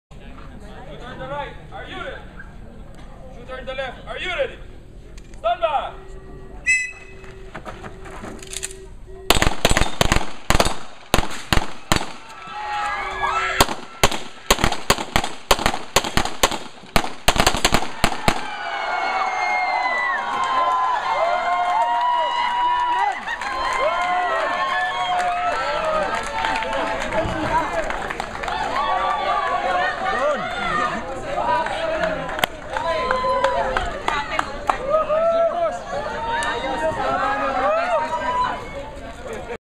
USEC Rico Puno Lev 3 Classic Division Shoot-Off Champion
Congratulations to Jr Shooter Jun for beating the top Philippine Shooters in the Usec Puno Cup Shoot off held at Armscor.